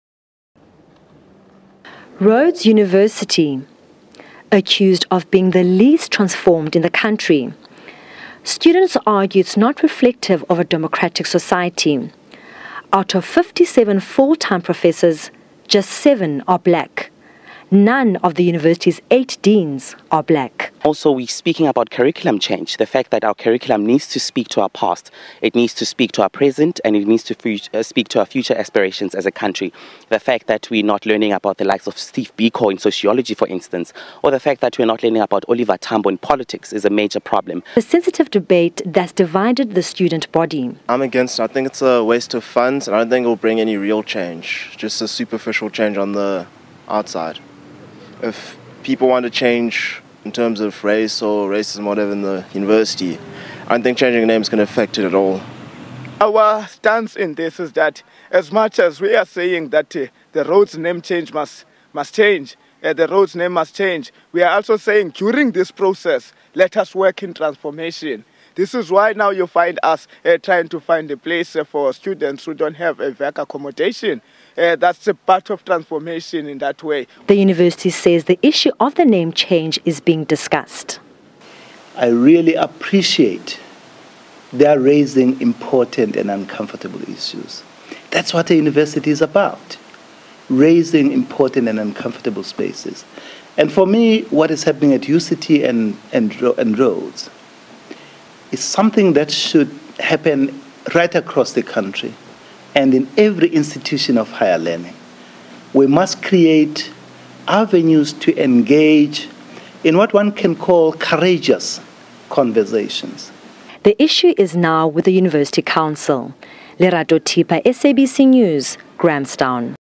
Rhodes University students call for urgent transformation of the institution
Rhodes University students are calling for urgent transformation of the institution. They also want a name change. The university is named after British imperialist Cecil John Rhodes. Students said he was a symbol of white supremacy and oppressed black people.